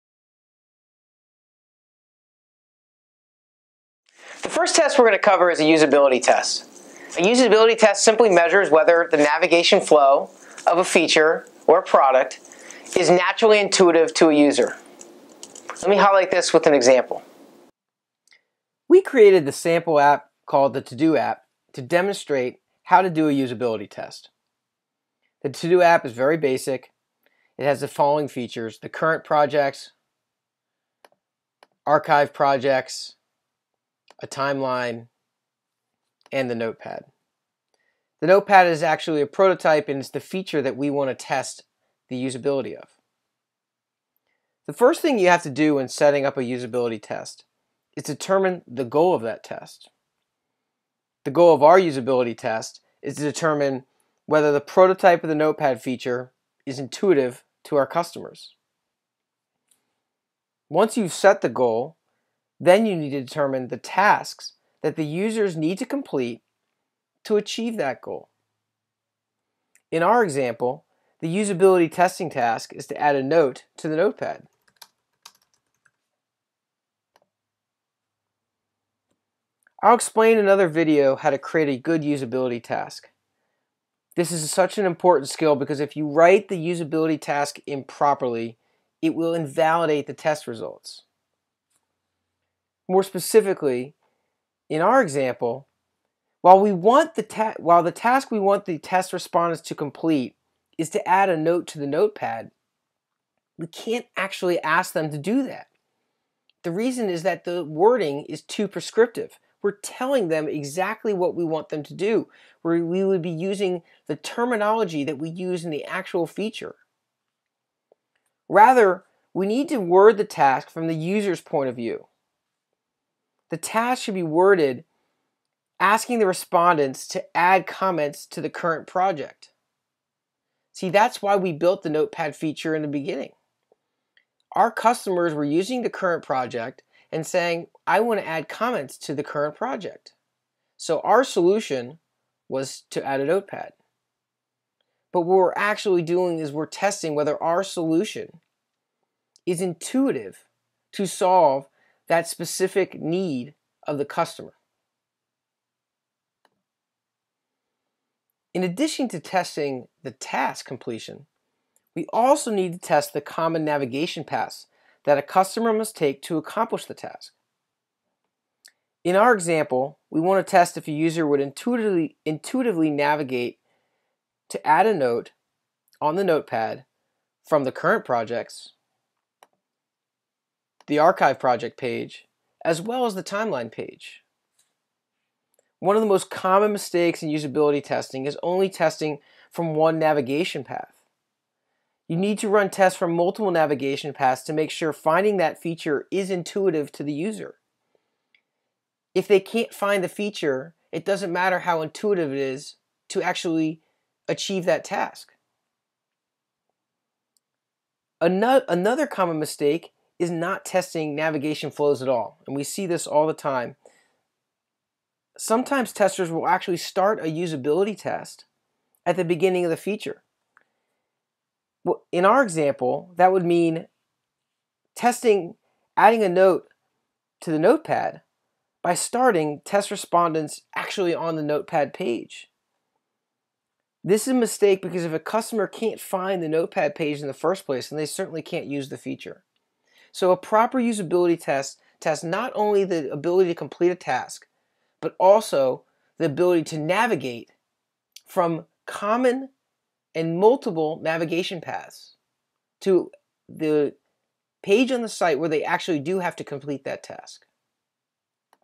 Usability Tests: Introduction
This is an introduction to why and how you conduct usability tests.